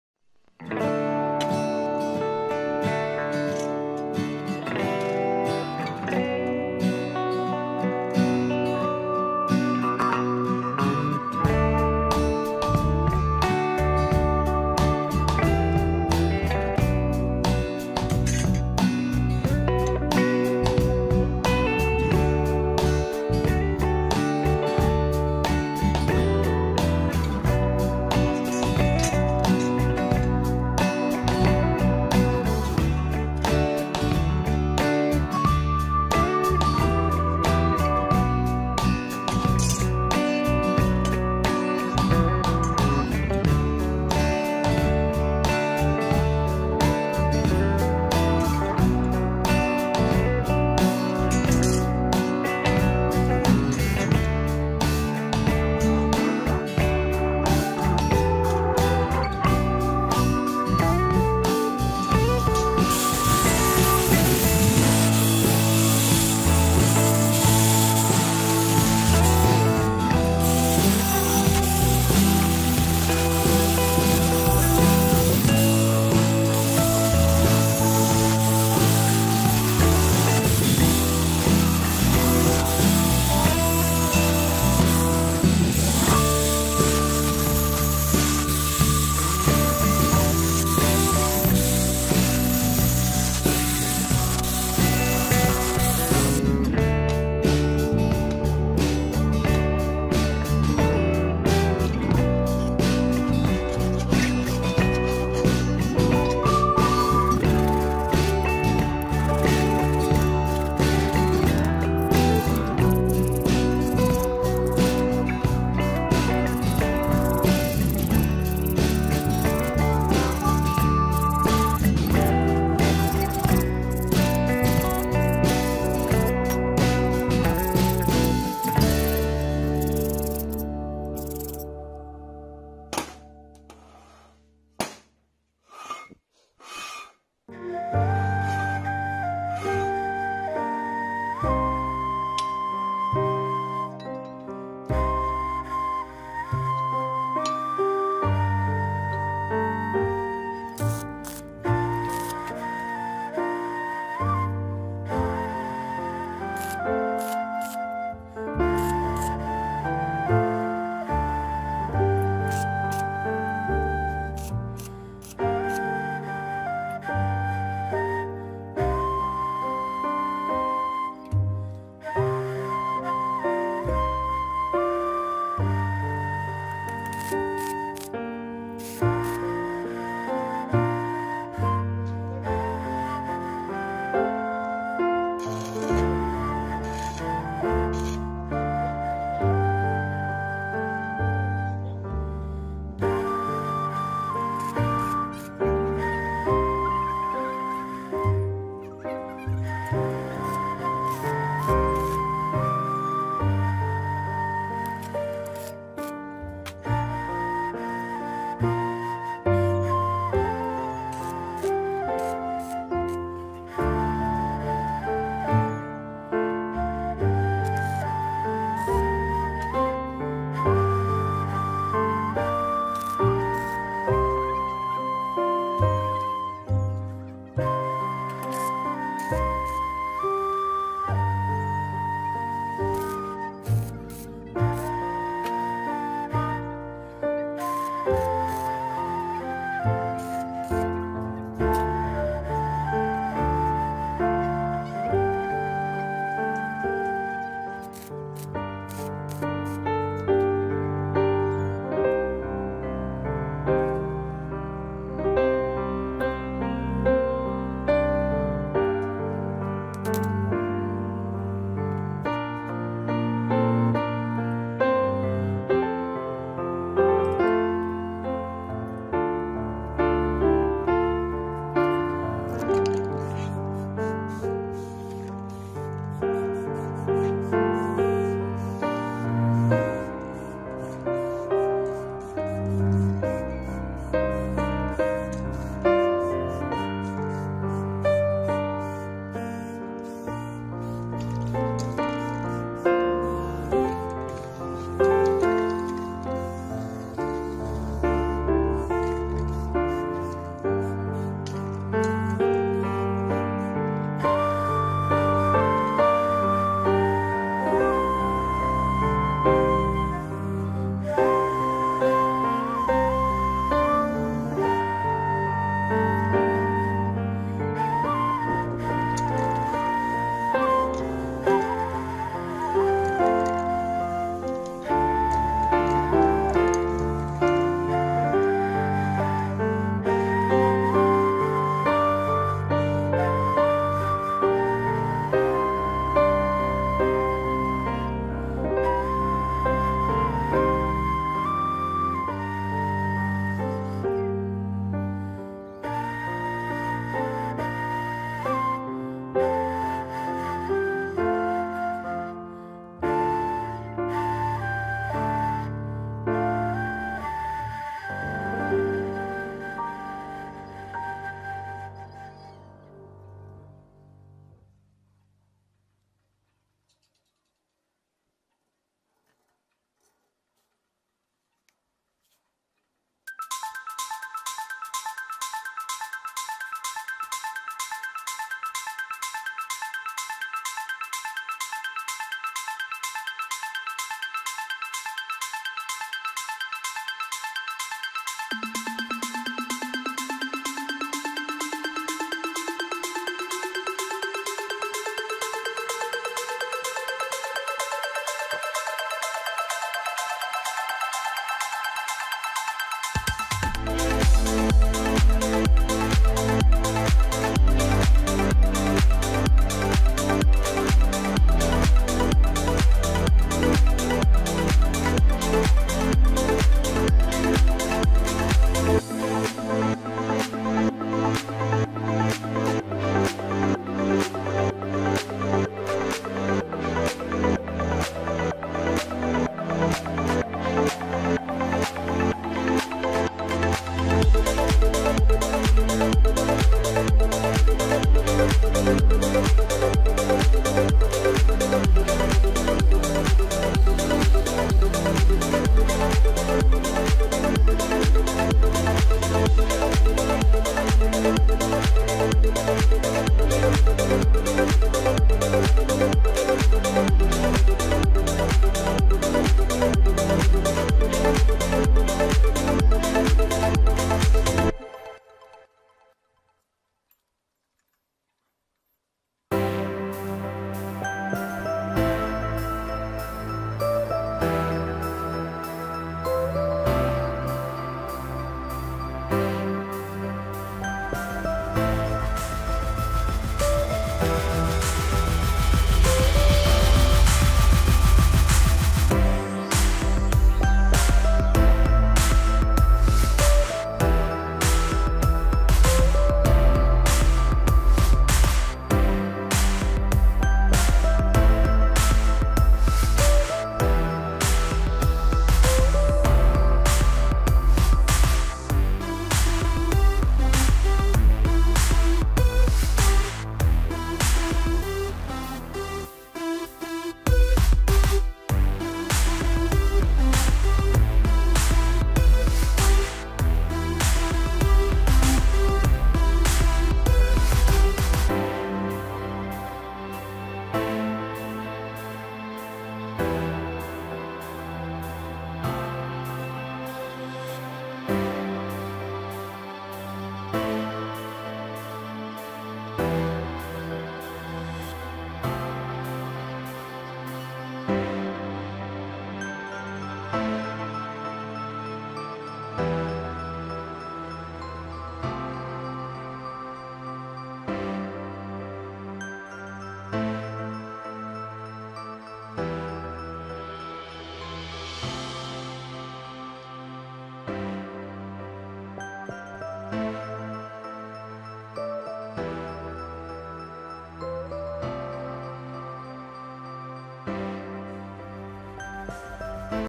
Design your own beautiful desk clock from scrap iron
I work in the mechanical field, for me it is not difficult to come up with ideas and implement my ideas. I love my job, I love watches, I will make watches from materials I have available. It's scrap iron, scrap iron
We look forward to receiving your support and encouragement